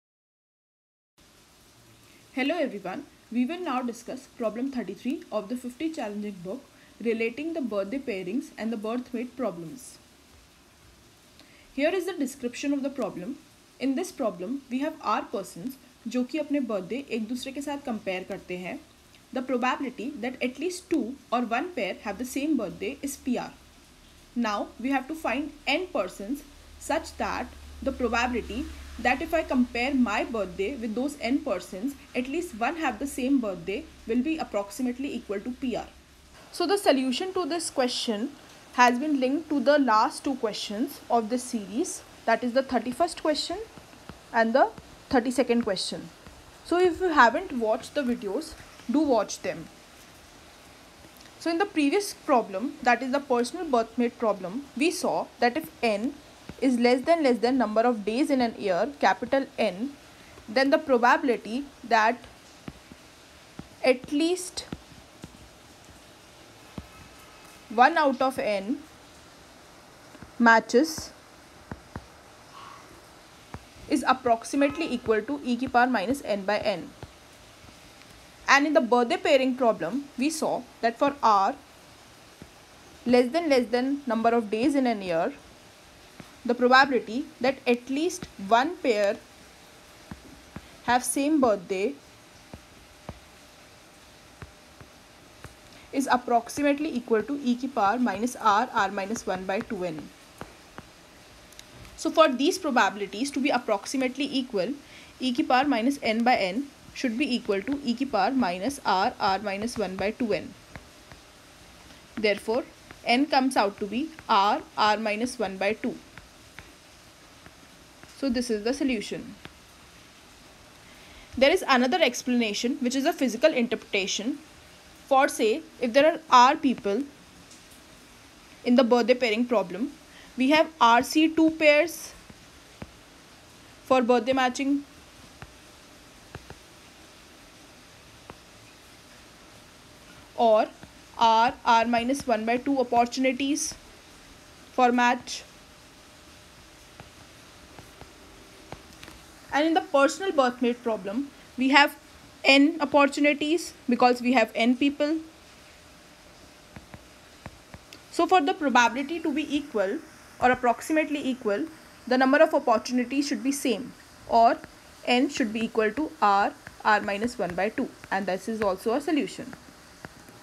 33 : Relating the Birthday Pairings and Birthmate Problems | Ace Quant | Probability Theory
Question 33 :
If r persons compare birthdays in the pairings problem, the probability is PR that at least 2 have the same birthday. What should n be in the personal birthmate problem to make your probability of success approximately PR ?